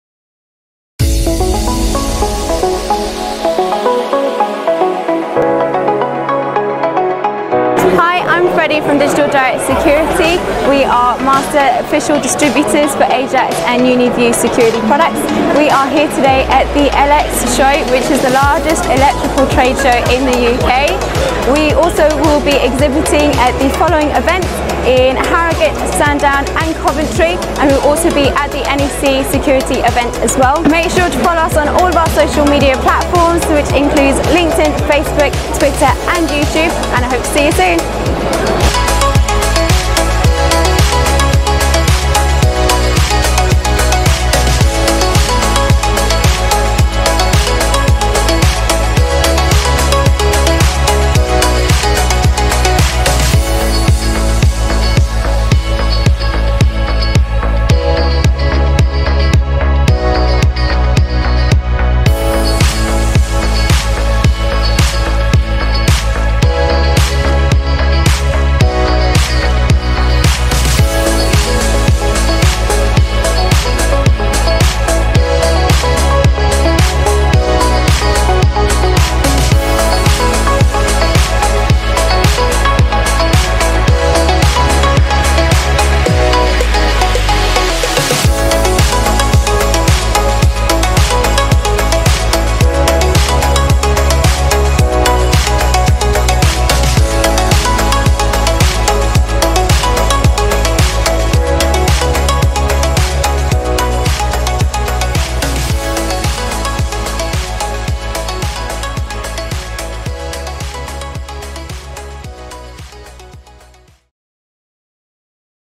ElexShow 2020 - Alexandra Palace - DDS - CCTV - Wireless Alarms
We would like to take the opportunity to thank all potential Clients and Business Partners for visiting our stand at Alexandra Palace - ElexShow 2020.

We hope you enjoyed your visit and the hospitality offered at our stand.